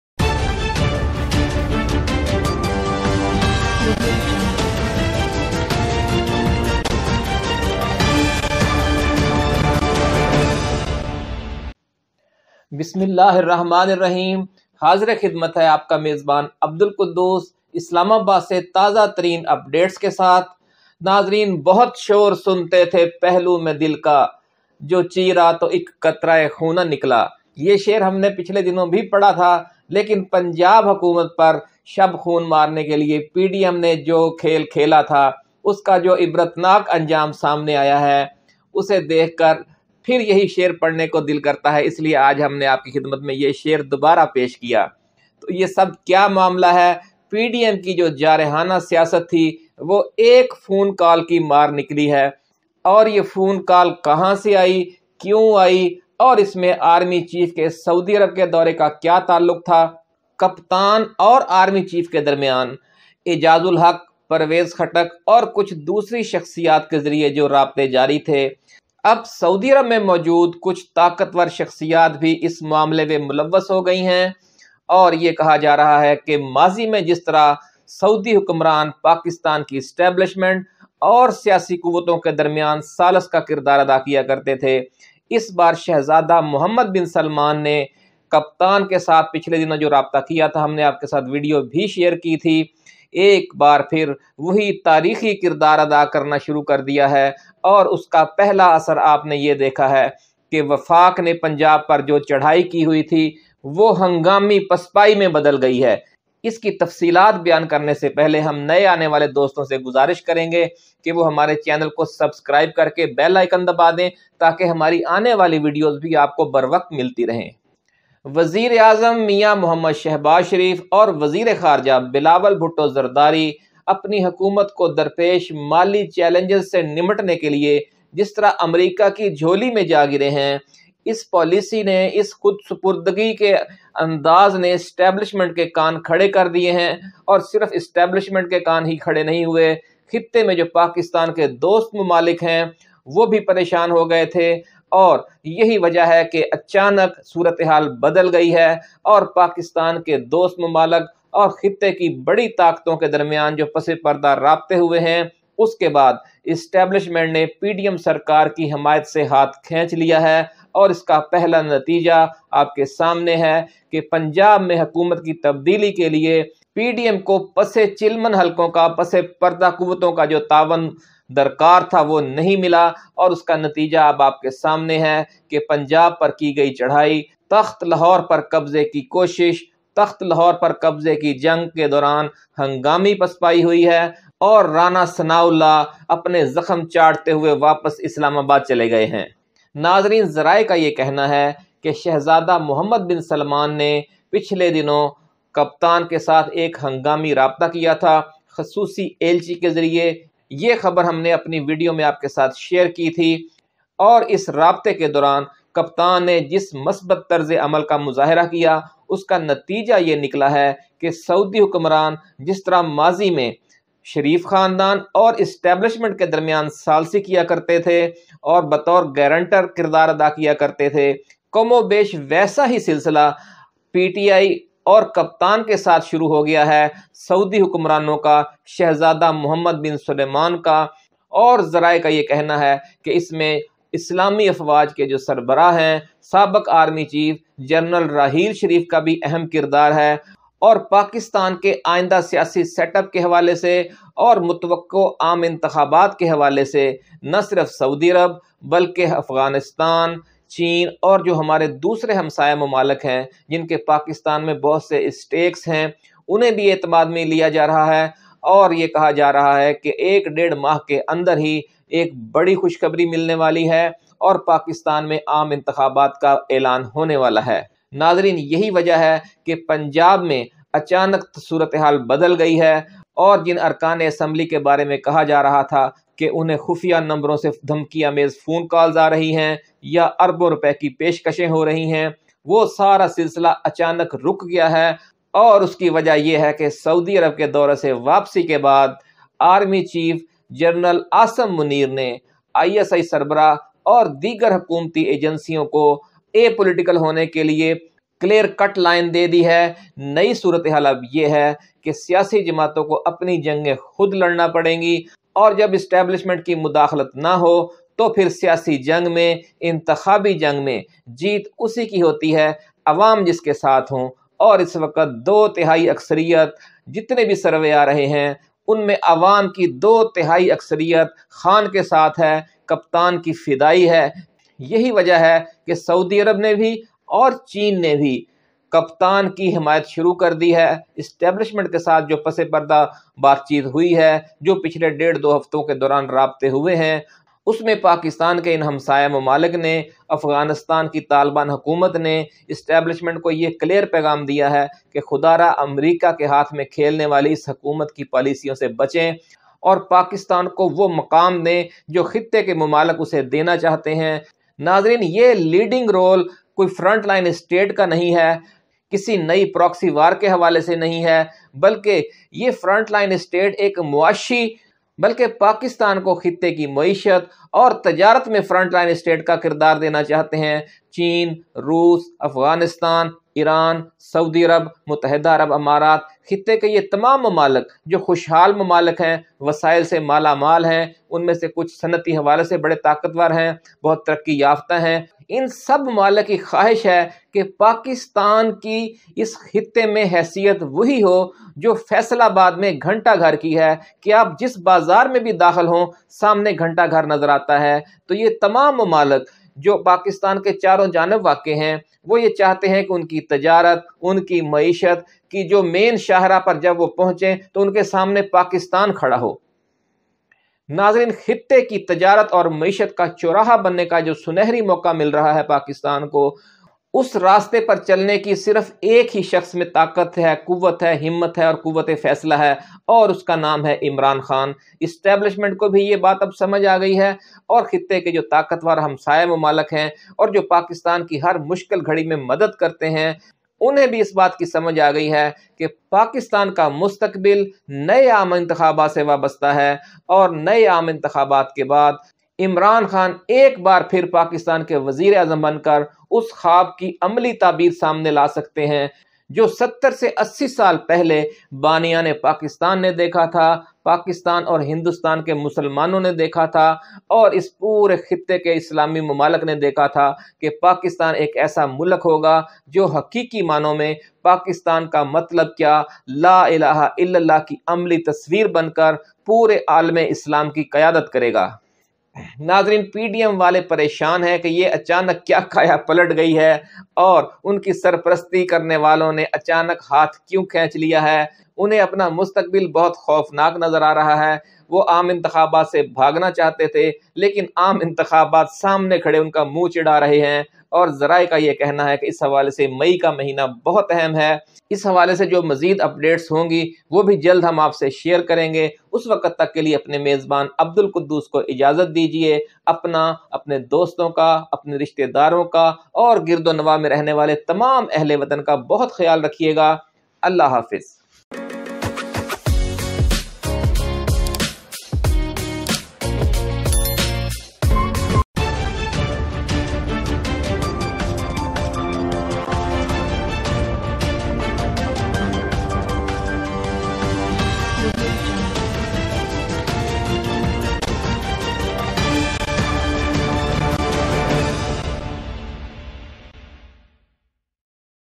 پنجاب پر چڑھائی ایک فون کال کی مار نکلی ، کپتان آرمی چیف رابطے ؟ شہزادہ محمد بن سلطان گارنٹر ؟
اسٹیبلشمنٹ نے شہبازحکومت کی سرپرستی سے اچانک ہاتھ کیوں کھینچ لیا ؟